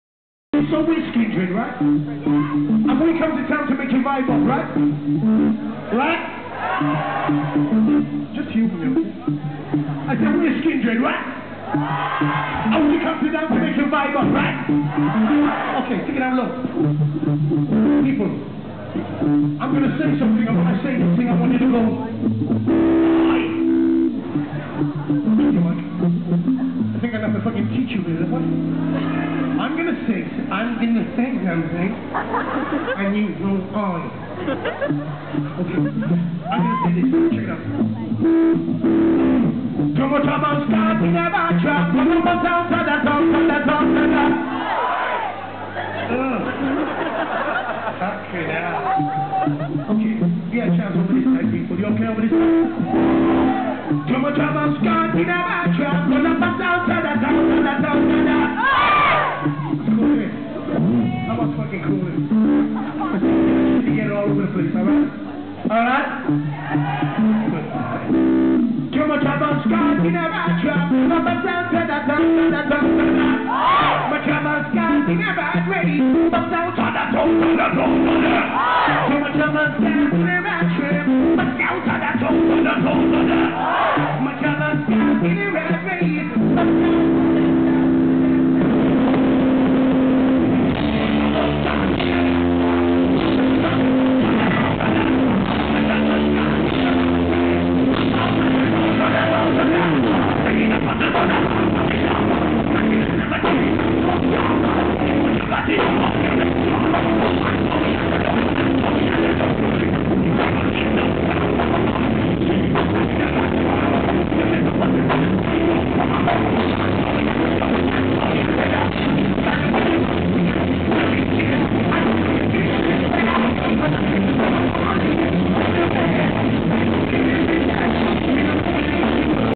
Skindred (supporting Gogol Bordello) 30/03/2008
Skindred at manchester academy supporting Gogol Bordello